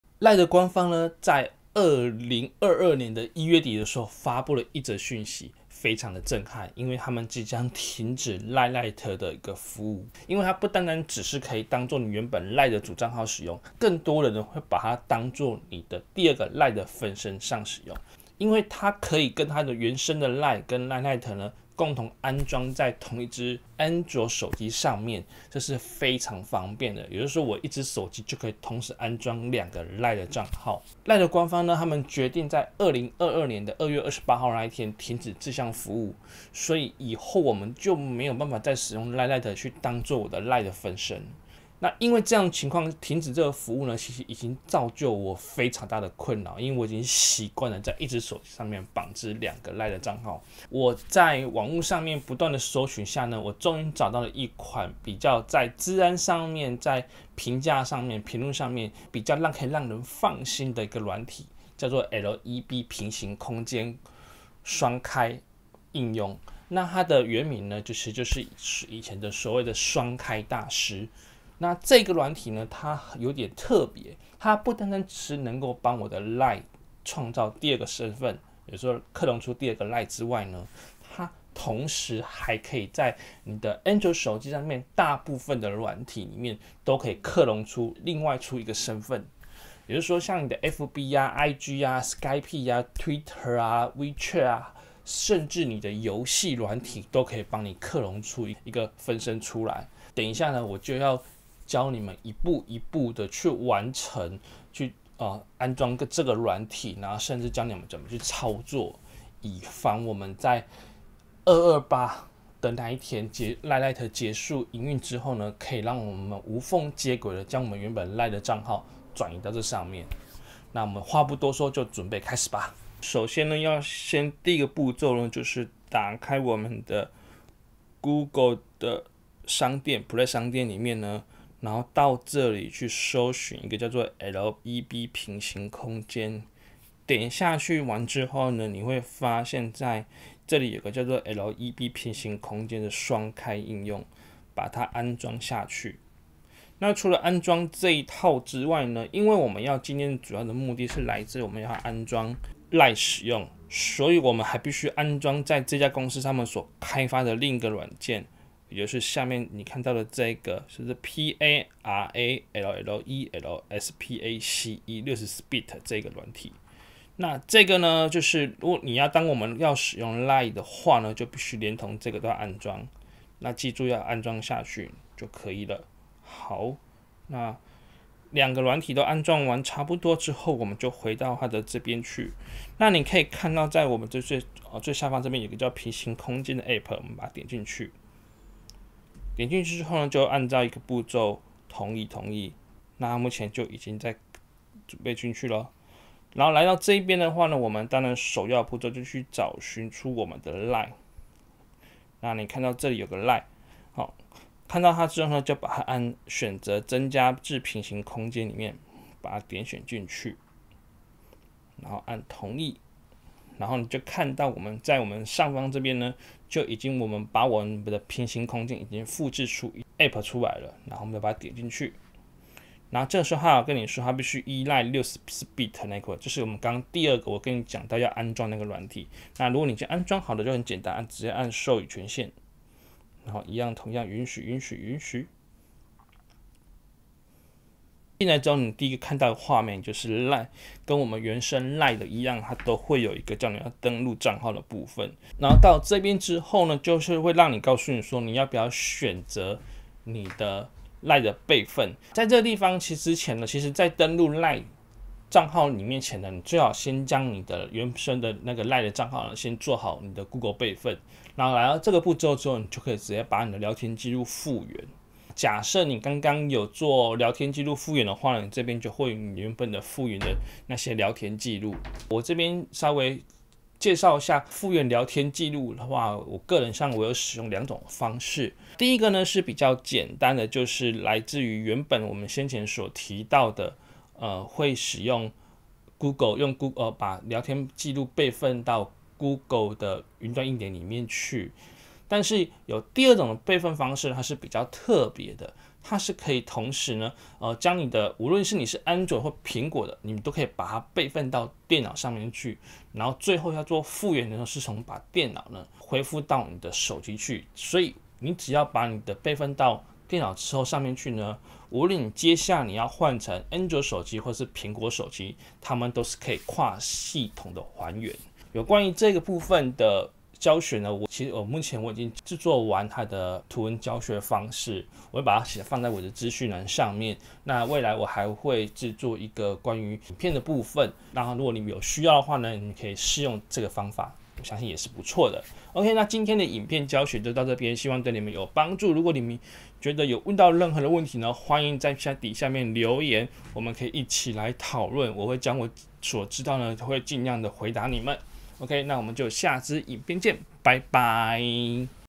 LINE雙開｜使用安卓手機開分身｜LINE一機雙帳讓你私帳、公帳訊息電話不漏接
LINE 官方表示 LINE Lite 將於日本時間2月28日0點停止服務，對於已經習慣使用 LINE Lite 當作雙開LINE帳號的人來說，可以說是非常頭痛，畢竟這是 LINE Lite 是來自官方自己出的 App，在資訊安全上還是比較可以讓人放心，LINE Lite 停止服務後變成只能找尋第三方 APP 來作為 LINE 雙開帳號使用，作為目前穩定性的第三方雙開功能，我會比較推薦這款 LBE平行空間雙開應用 （又稱 雙開大師 ）。

00:00 LINE lite 停止服務
00:52 替代LINE lite方案
01:02 LEB平行空間雙開應用
02:03 雙開大師操作教學
04:32 聊天訊息備份方法